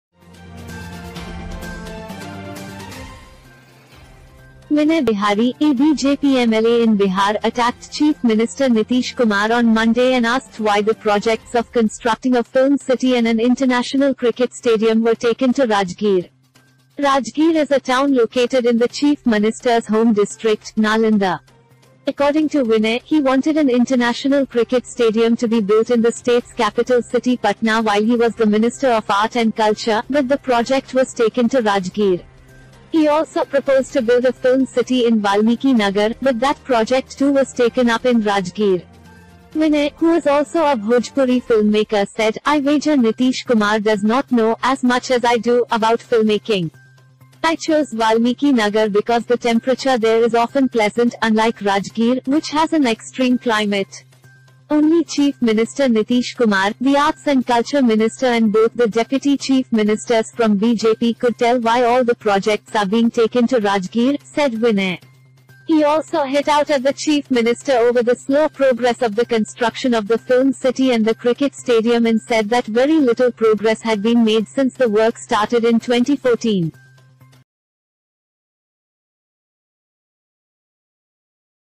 ‘Why are all the projects only in Rajgir’: BJP MLA attacks Bihar CM Nitish Kumar
0:10 ‘Why are all the projects only in Rajgir’: BJP MLA attacks Bihar CM Nitish Kumar